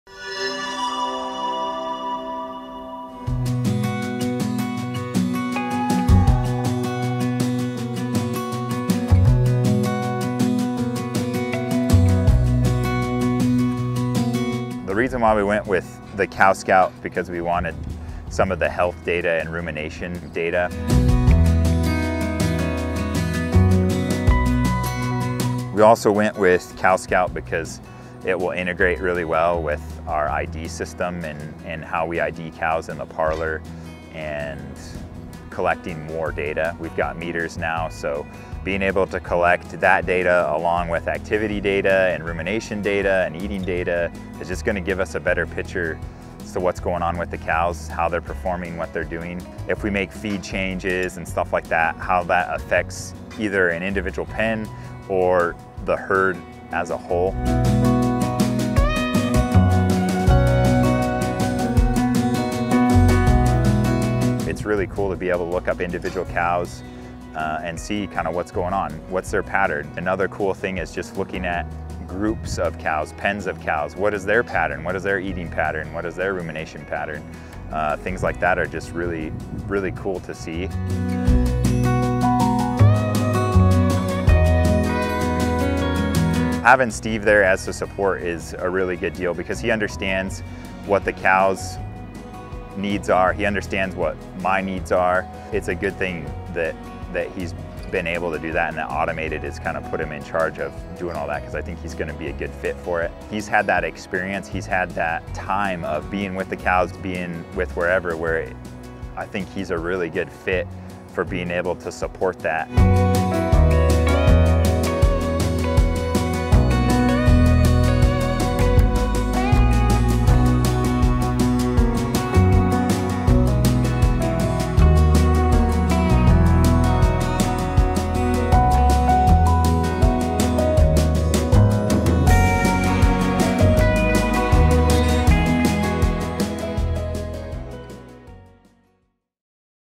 CowScout- Liberty Jersey Farm
Automated Dairy installed 600 GEA CowScout collars at Liberty Jersey Farm in Fallon, NV.